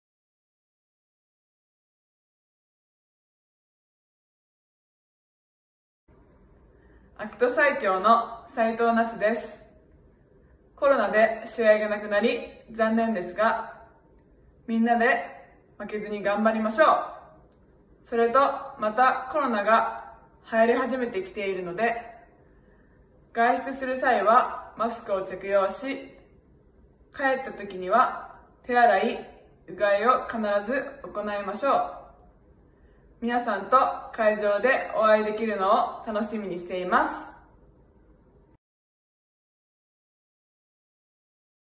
【mybadminton】#42 齋藤 夏 選手（ACTSAIKYO所属）
全てのバドミントンプレイヤーの皆さんへバドミントン日本代表メンバー BIRDJAPAN の選手たちからメッセージをお届け。
今回はB代表混合ダブルスの齋藤夏選手です。

2020年8月12日公開